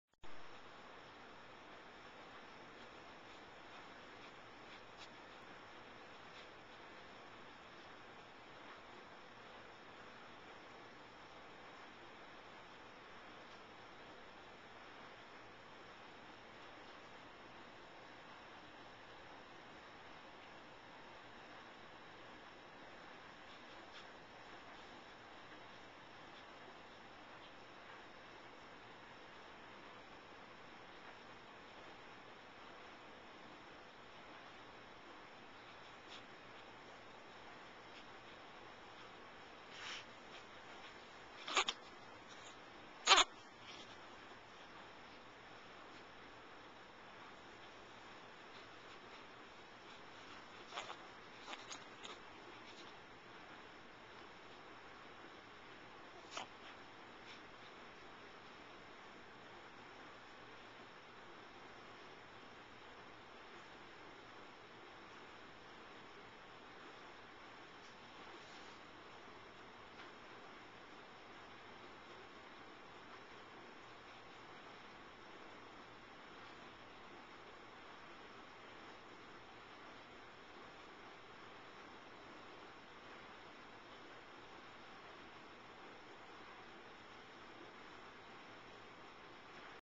Baby Xander holds Mommy's hand.
Xander hanging on to Mom's hand.  OK, Mom's finger.